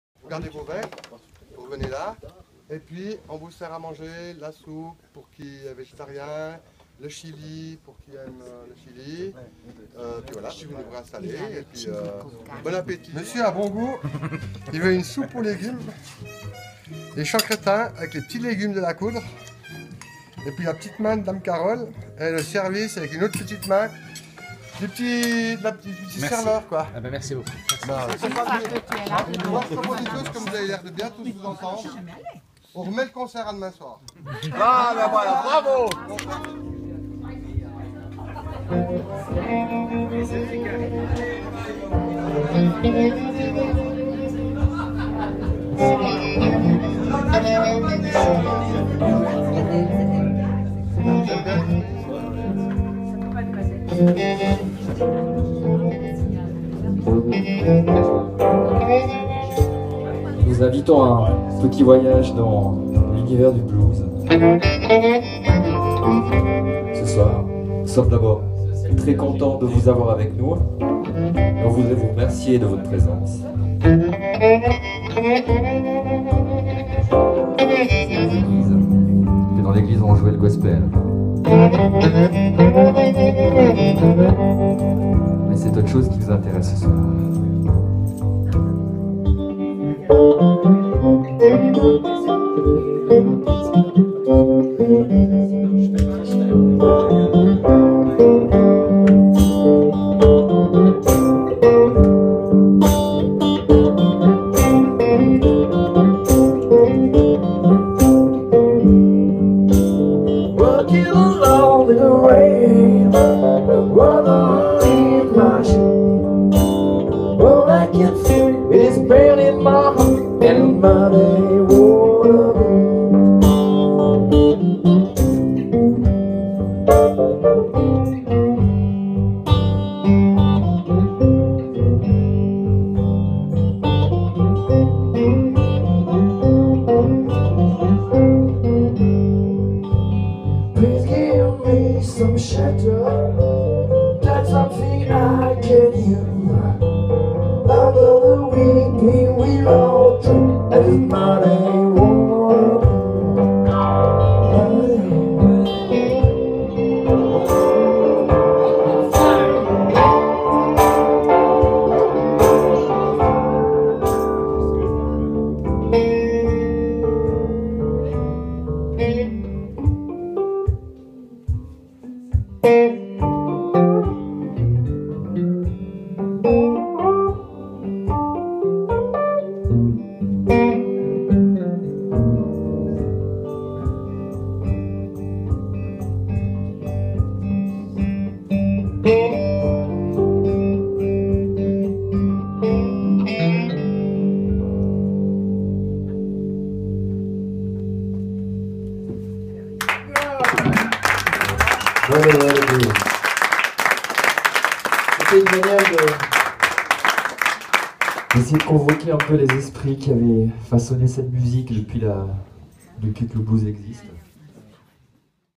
les Z-Bros a La Table de Carole
A very unique and private evening with 50 guests and wonderful performance by les Z-Bros, Olivier Magarotto & Didier Chammartin, le duo Z-Bros fait revive l'ambiance des juke joint ou allait se perdre l'âme des esclaves pris dans les filets de la musique du diable. Thank you Vincent et Carole "La Table de Carole" Champs Cretins